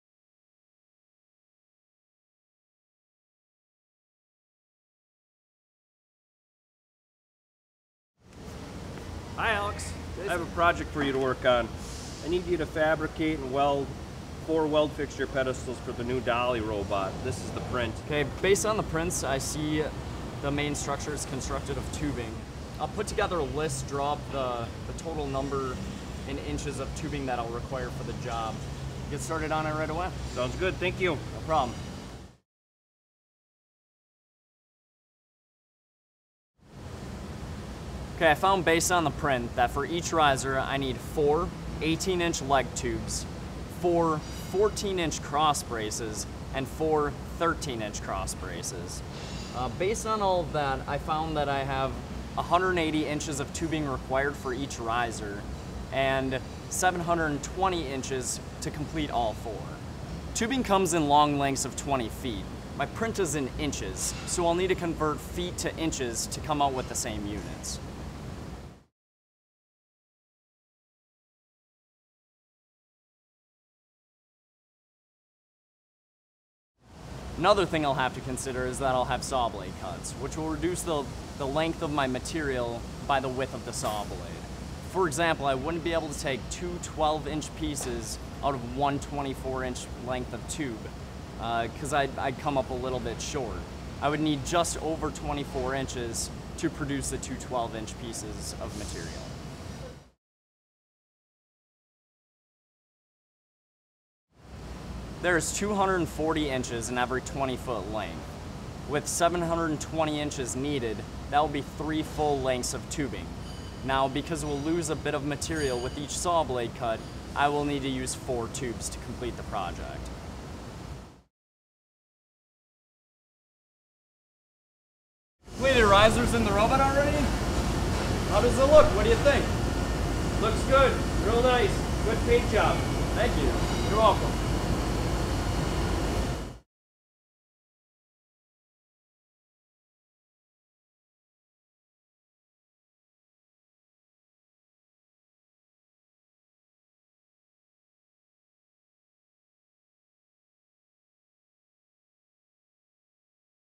GRM KI WHOLE NUMBERS
The NEW Manufacturing Alliance's 'Get Real Math' videos filmed at KI in Green Bay.  The manufacturing-related math problems showcase the following Common Core Skills:
4.OA  Use the four operations with whole numbers to solve problems.
4.MD Solve problems involving measurement and conversion of measurements.
7.G Draw construct, and describe geometrical figures and describe the relationships between them.